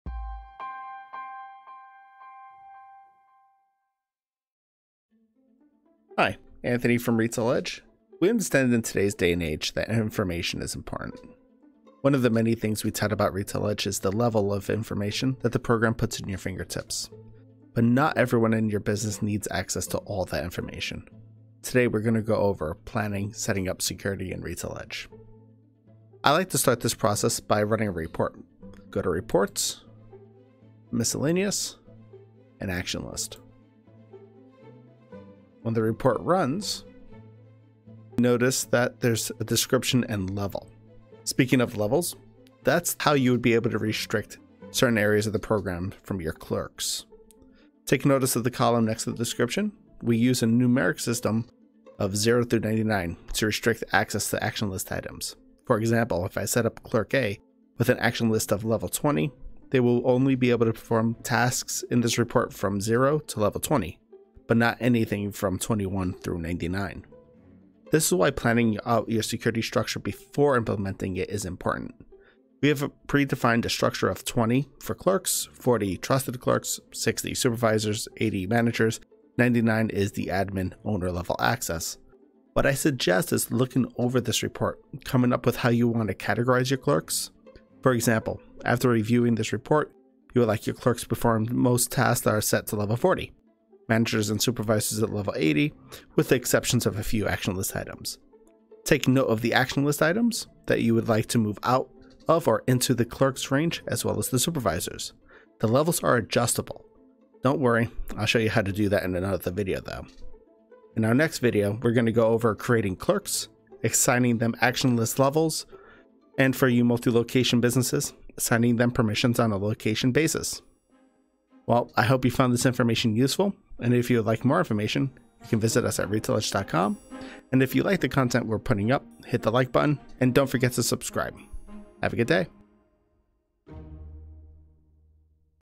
Planning Security in RetailEdge (1 of 3)
Internal POS security plays an important role in protecting your business: Some employees shouldn't have the same access as managers or owners. This part 1 of 3 video shows you the different actions in RetailEdge you can restrict. Some of these actions include:
-Editing a customer discount
-Mailing labels
-Issuing store credit or a gift card
-Editing an item

In the next part of this Security playlist, Anthony shows you how to set up numeric security levels for both action items and clerks. This is so clerks with a security level of 20 can perform actions that have a security level of 1-20, but cannot perform actions with a security level of 21-99.